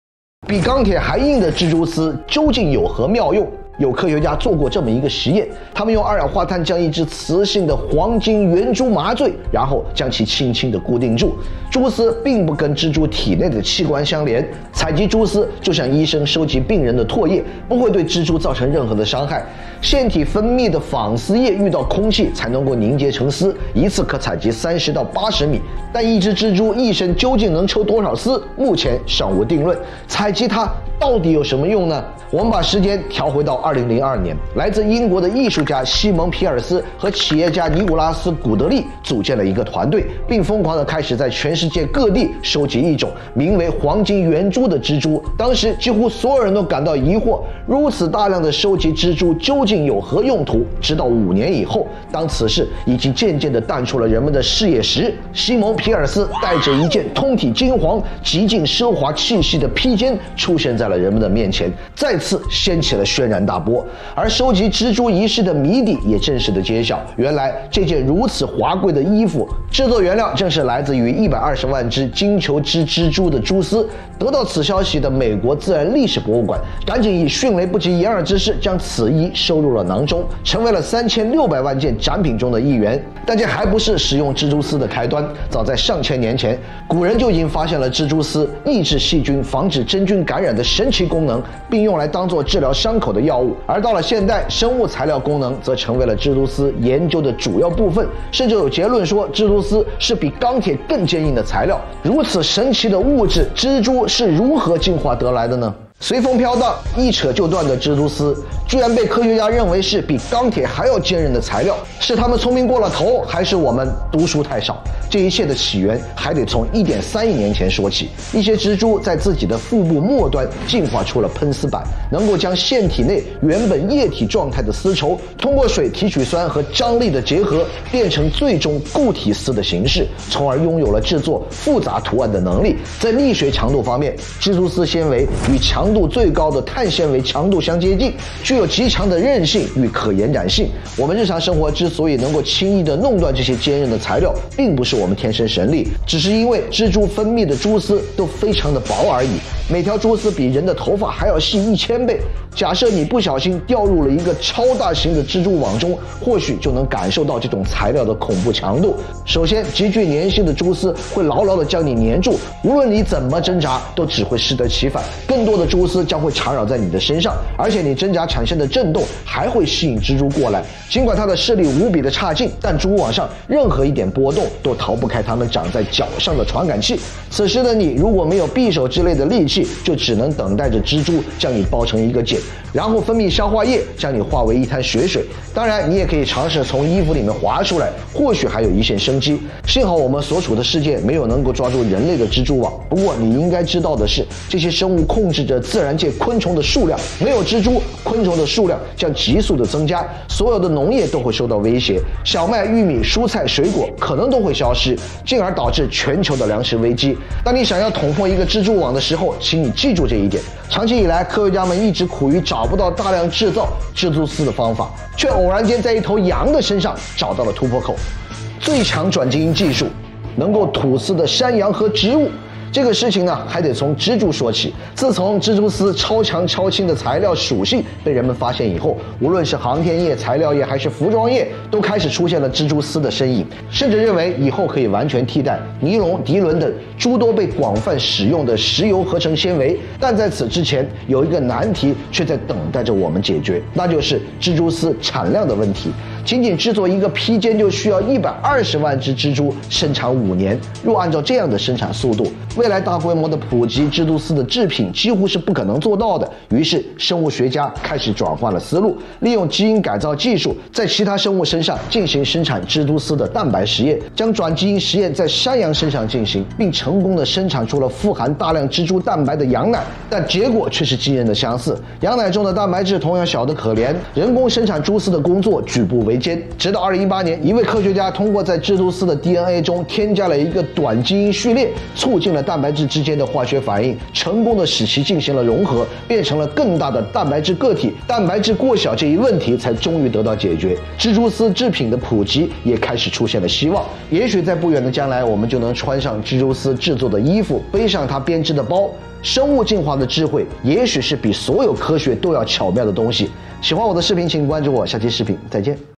蜘蛛丝，改变人类命运的黑科技
比钢铁还硬的蜘蛛丝，究竟有何妙用？